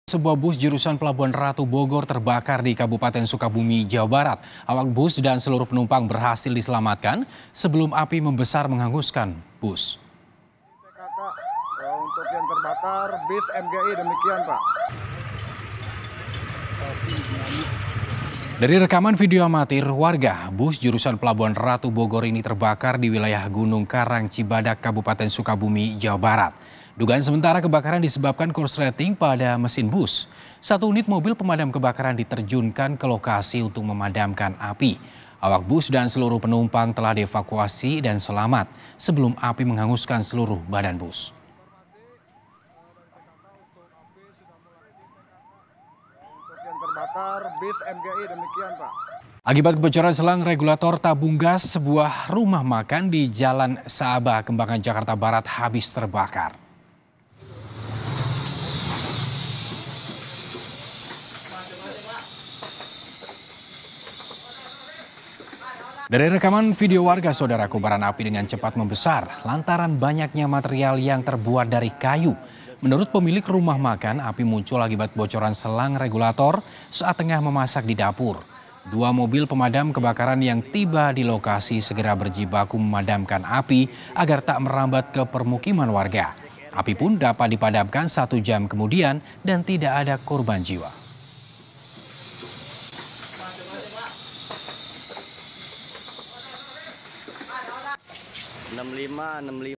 Bus Jurusan Pelabuhan Ratu-Bogor Terbakar, Begini Usaha Damkar Padamkan Api
SUKABUMI, KOMPAS.TV - Sebuah bus jurusan Pelabuhan Ratu - Bogor terbakar di Kabupaten Sukabumi, Jawa Barat.

Awak bus dan seluruh penumpang berhasil diselamatkan sebelum api membesar menghanguskan bus.

Dari rekaman video amatir warga, bus jurusan Pelabuhan Ratu - Bogor ini terbakar di wilayah Gunung Karang, Cibadak, Kabupaten Sukabumi, Jawa Barat. Dugaan sementara, kebakaran disebabkan korsleting pada mesin bus.

Satu unit mobil pemadam kebakaran diterjunkan ke lokasi untuk memadamkan api.

Awak bus dan seluruh penumpang telah dievakuasi dan selamat sebelum api menghanguskan seluruh badan bus.

Baca Juga Gubuk Tempat Tinggal Penjaga Taman Kompleks Angkasa Pura Terbakar, Diduga Karena Korsleting Listrik di